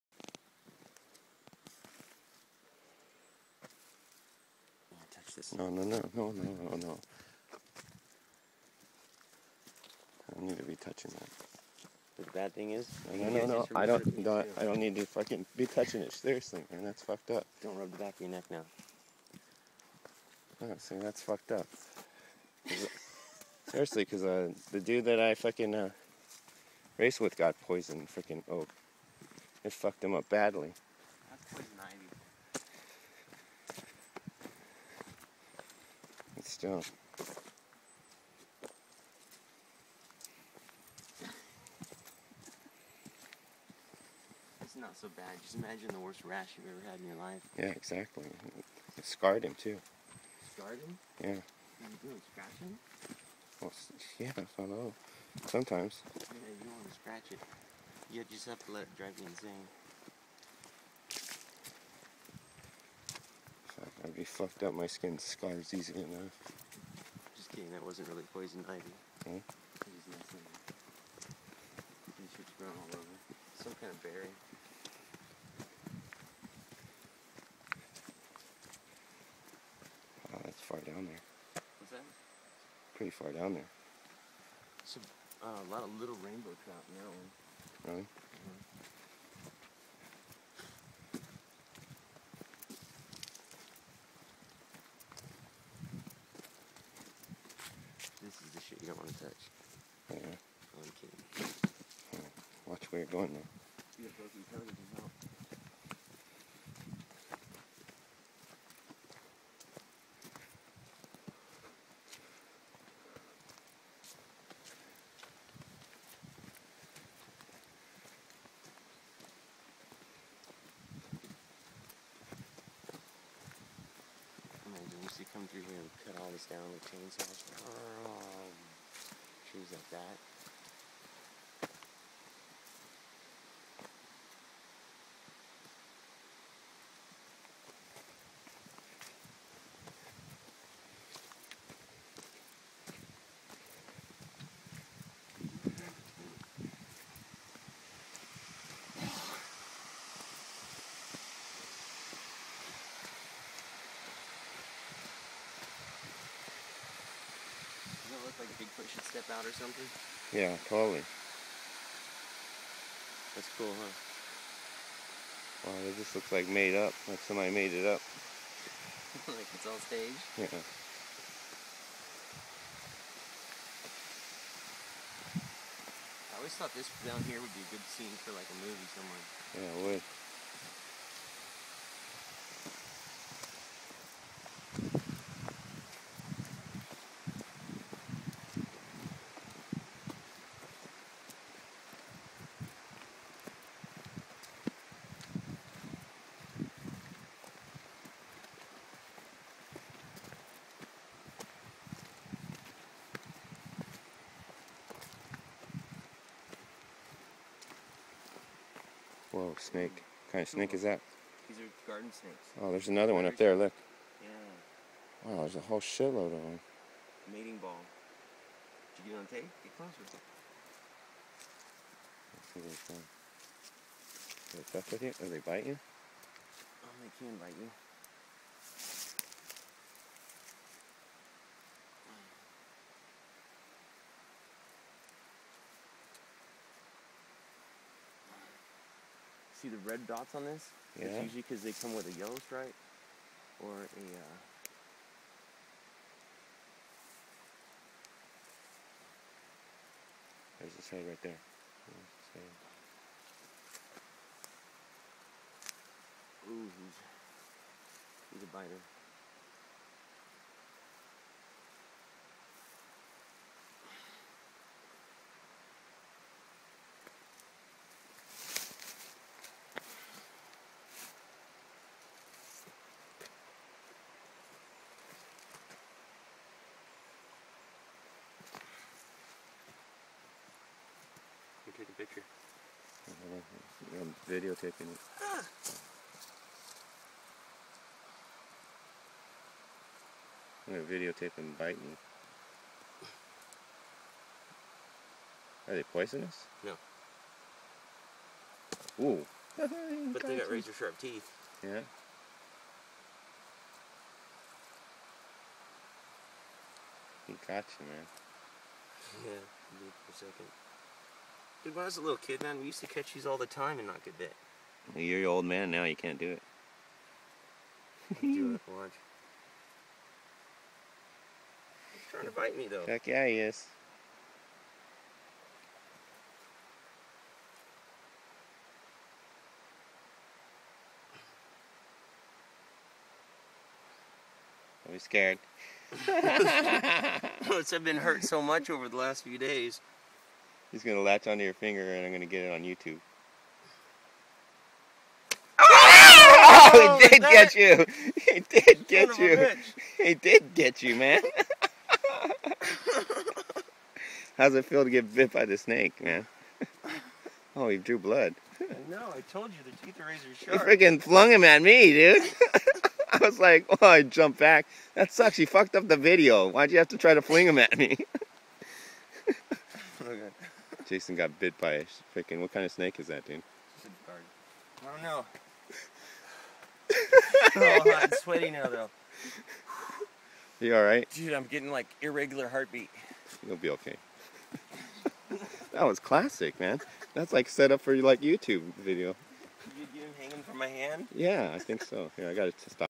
Bite from snake
Snake bite, trust me the snake was much larger than it looks!